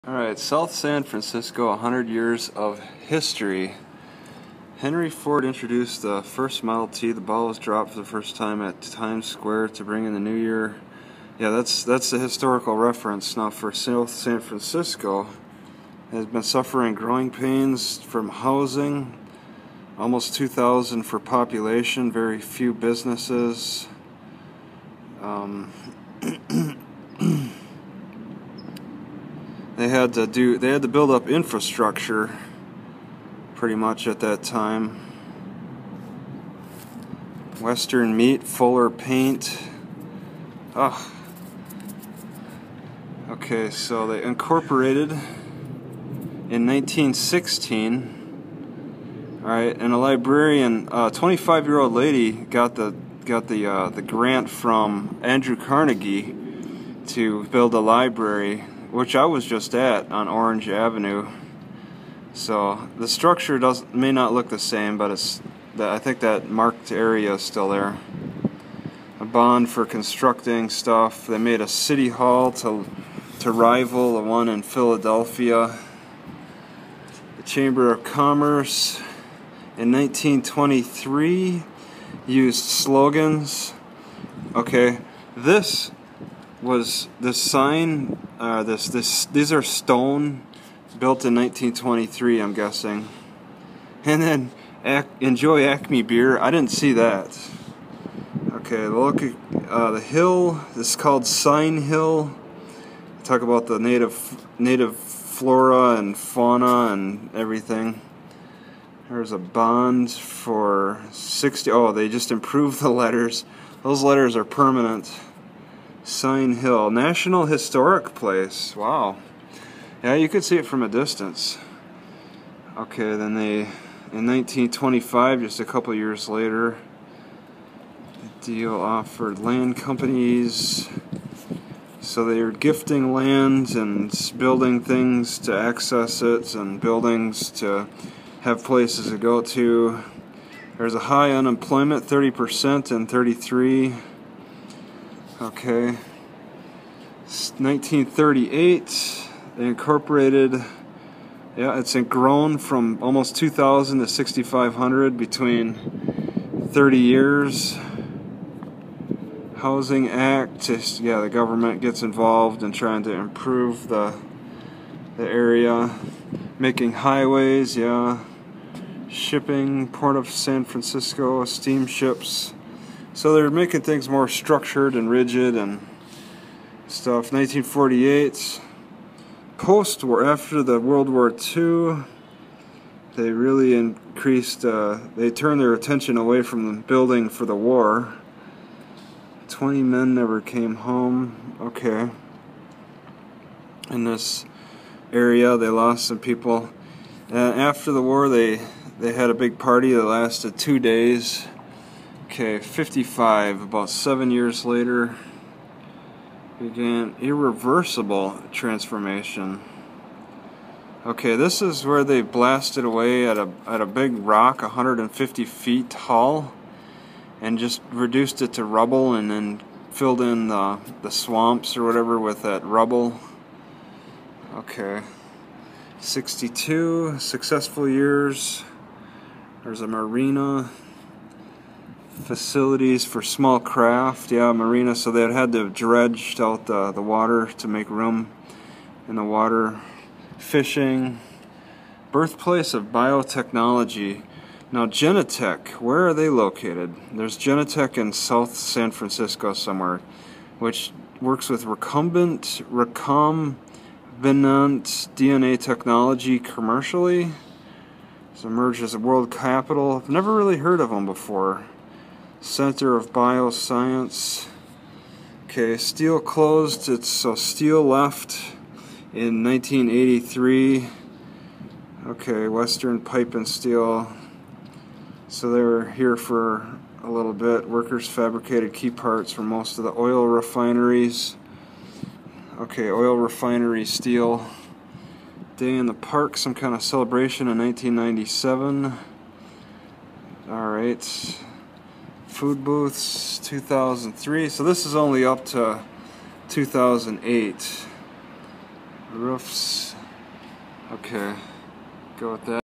Short Read of History of South San Francisco 1908-2008; at Museum; California
Well-detailed history of the South San Francisco area. You can walk away from this mural understanding why the city has formed like it did. Actually, I'm kidding. It made no mention of the Chinese influence which appears to be very strong in the area. Don't tell me that the Chinese arrived in droves after 2008, because they appear to be very established in the area. They are enjoying amazing climate in the area. This isn't even "China Town" either, as far as I understand.

If you study Feng Shui, there is probably some information on how Chinese view natural climate. They always seek balance in their beautiful lives and probably consider a very stable climate to be a great omen for balanced living. But who wouldn't?